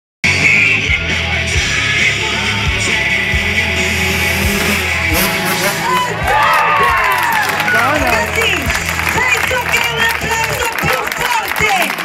Salto con la moto
un bel saltino filmato da me il 4-7-09 nel parcheggio dello stadio di calcio a Novara